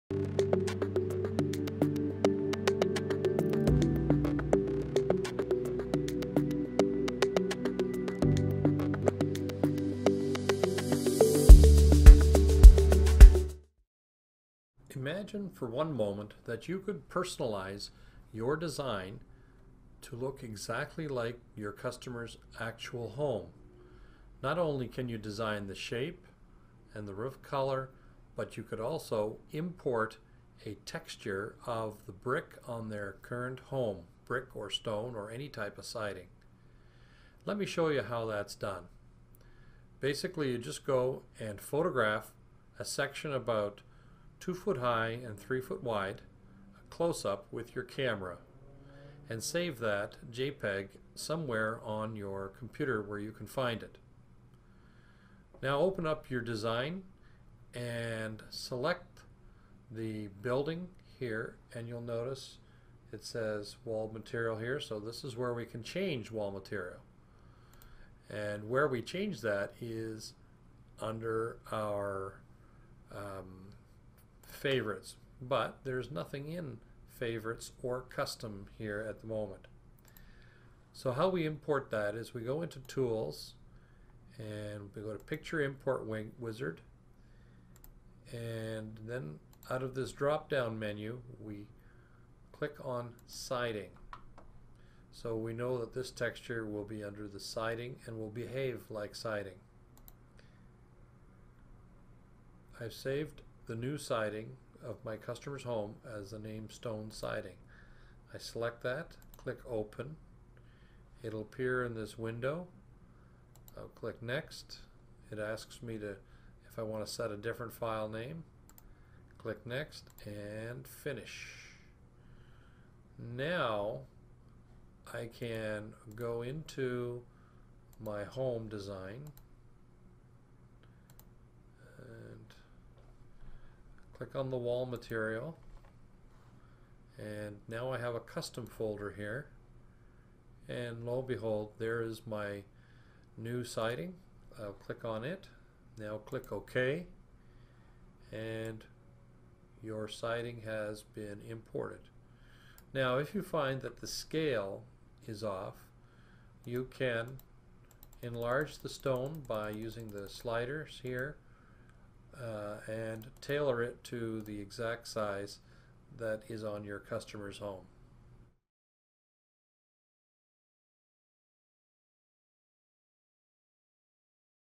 How To Import Your Customer's Exterior Wall Texture into UVision
𝗪𝐚𝐧𝐭 𝐭𝐨 𝐥𝐞𝐚𝐫𝐧 𝐦𝐨𝐫𝐞? 𝐕𝐢𝐬𝐢𝐭 𝐭𝐡𝐞 𝐅𝐀𝐐𝐬 𝐬𝐞𝐜𝐭𝐢𝐨𝐧

𝐅𝐨𝐥𝐥𝐨𝐰 𝐮𝐬 𝐟𝐨𝐫 𝐦𝐨𝐫𝐞 𝐠𝐫𝐞𝐚𝐭 𝐜𝐨𝐧𝐭𝐞𝐧𝐭 𝐚𝐧𝐝 𝐢𝐧𝐬𝐩𝐢𝐫𝐚𝐭𝐢𝐨𝐧!